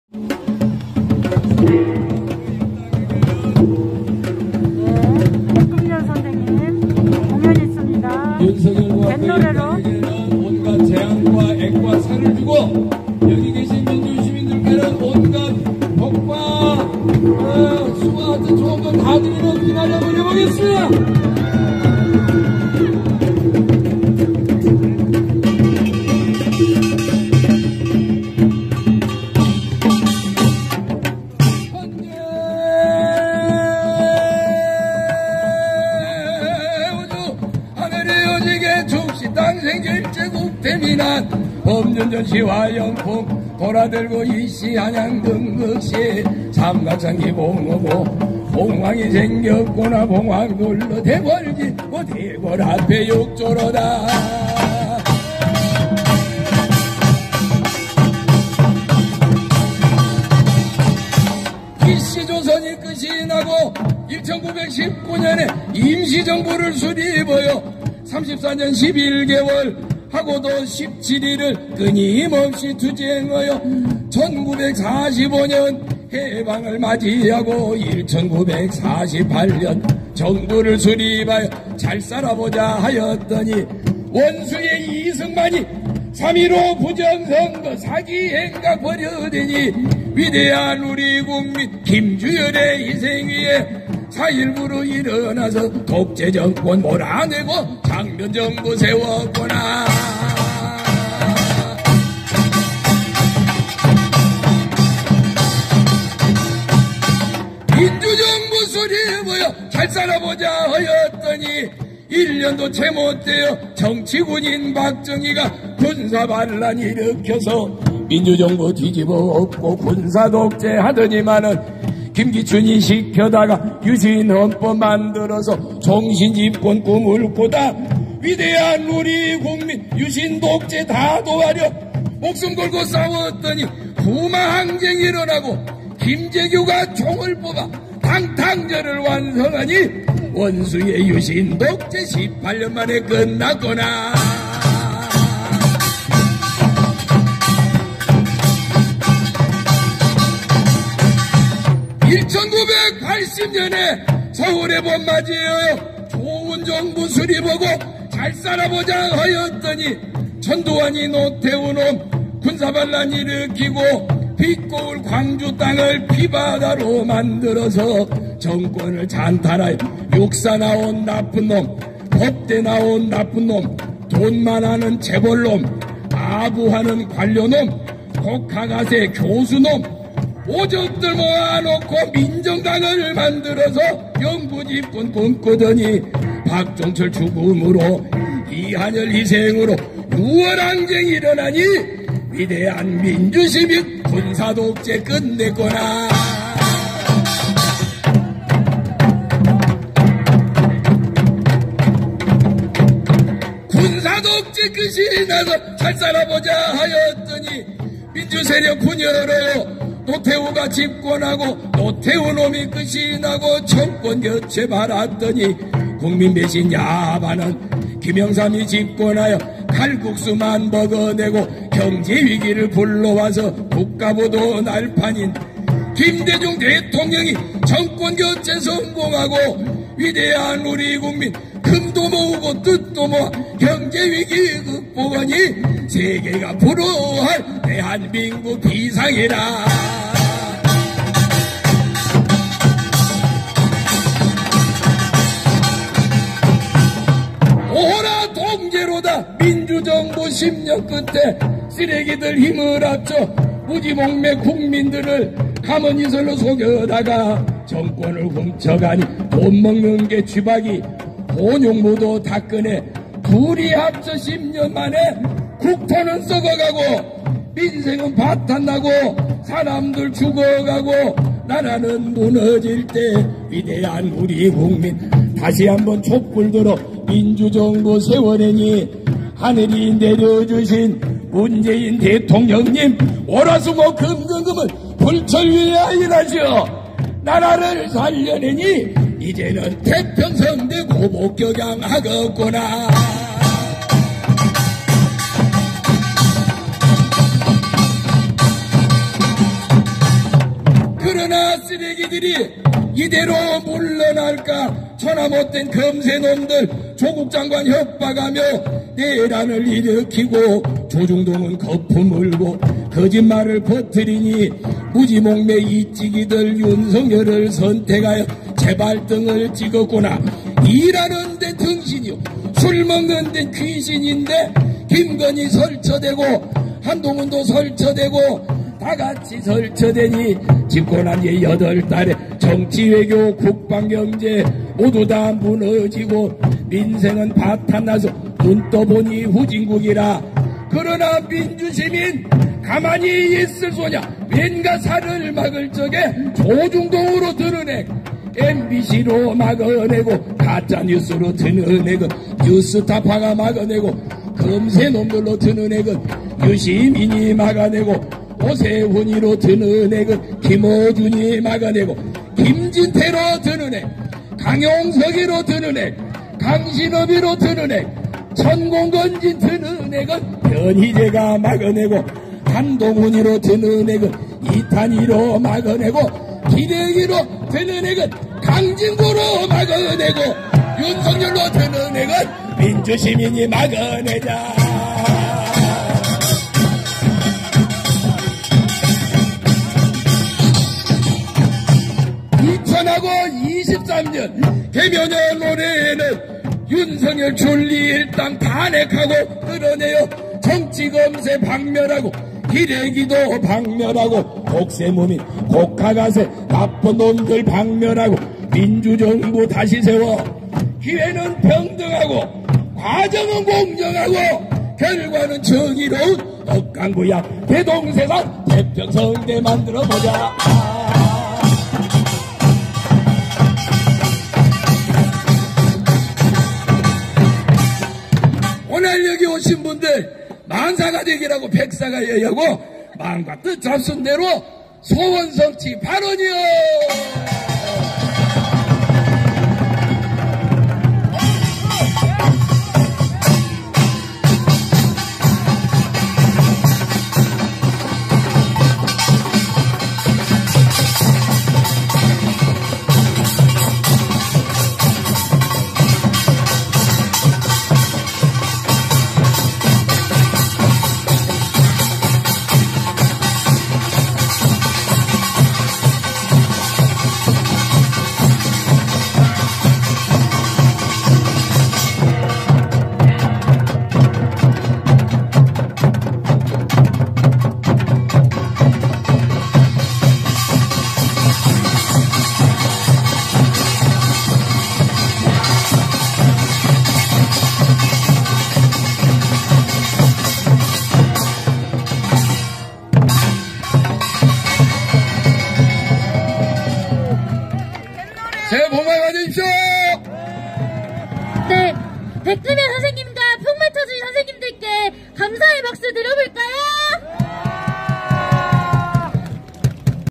[230107] 광주촛불, 백금렬과 속이 뻥 뚫리는 굿거리장단 한 판!!
* 더탐사팀 광주 촛불대행진 참여